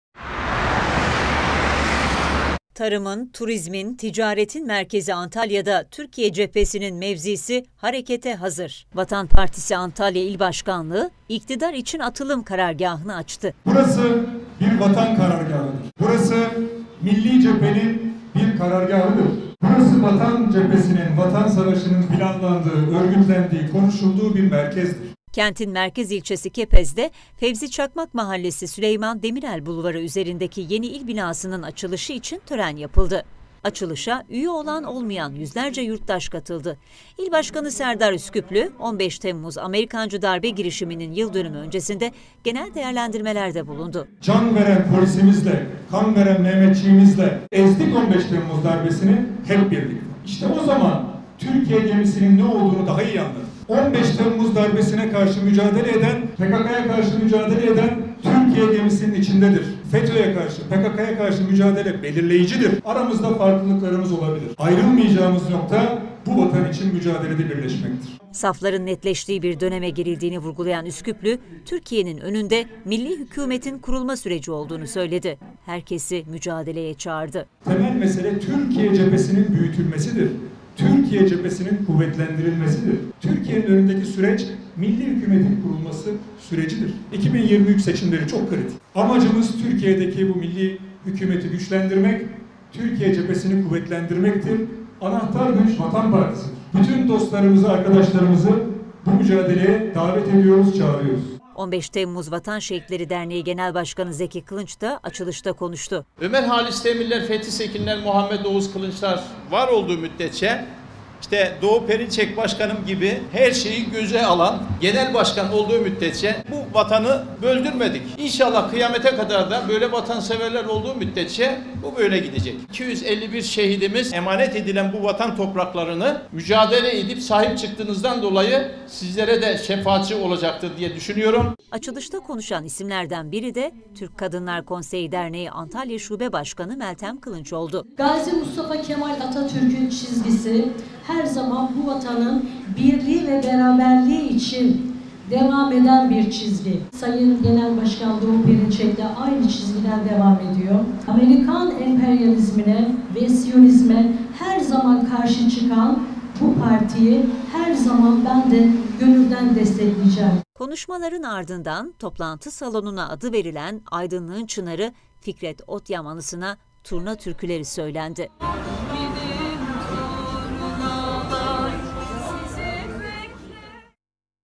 Vatan Partisi Antalya’da Türkiye cephesinin karargâhını açtı
Vatan Partisi Antalya’da Türkiye cephesinin karargâhını açtı. Merkez Kepez ilçesindeki yeni il binasının açılışına katılım yoğun oldu. Açılışta toplantı salonuna adı verilen Aydınlık’ın çınarı Fikret Otyam anısına turna türküleri söylendi.